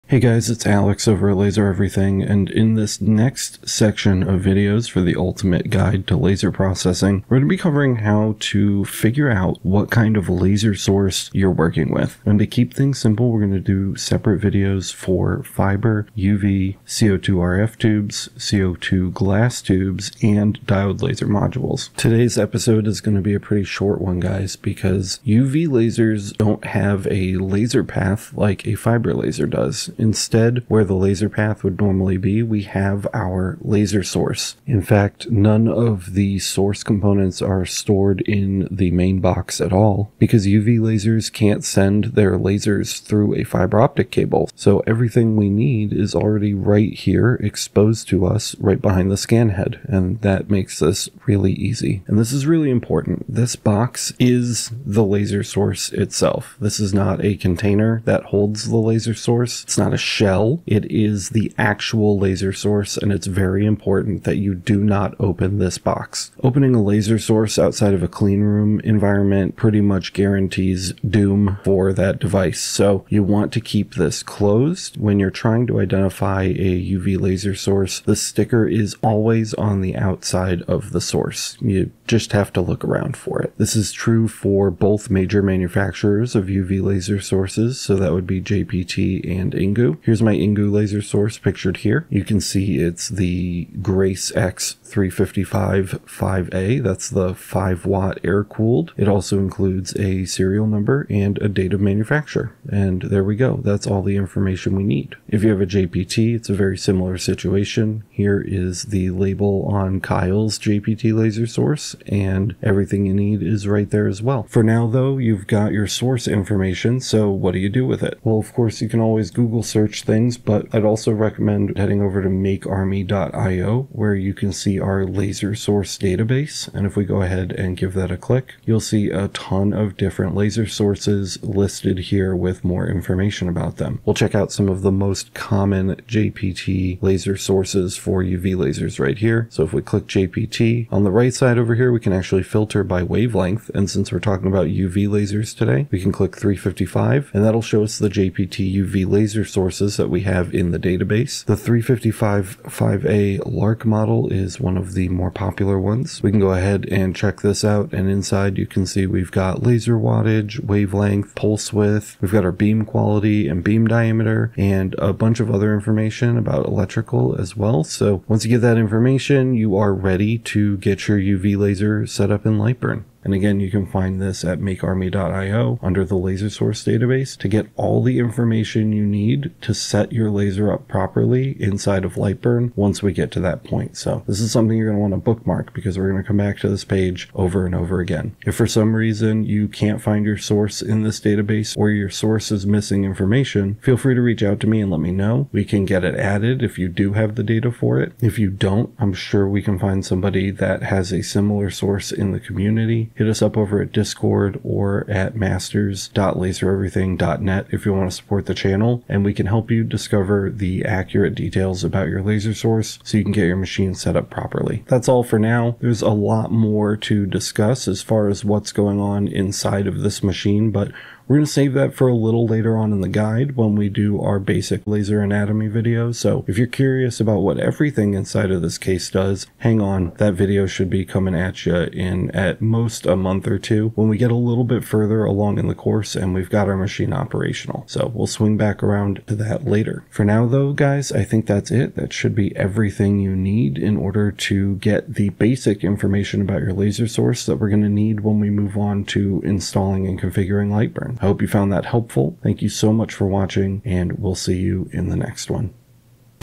What Laser Do YOU Have? Identifying UV Lasers
In this episode, we show you how to identify your UV laser source the right way. Whether you’re new to UV lasers or just curious about what’s inside your machine, we've got you covered. Knowing your source is critical for troubleshooting, upgrades, and safety compliance. If you’ve ever wondered what’s powering your UV laser, this guide will walk you through the essentials.

Need to submit a missing source or have files or documentation to share?
Is an existing entry missing information? Let us know!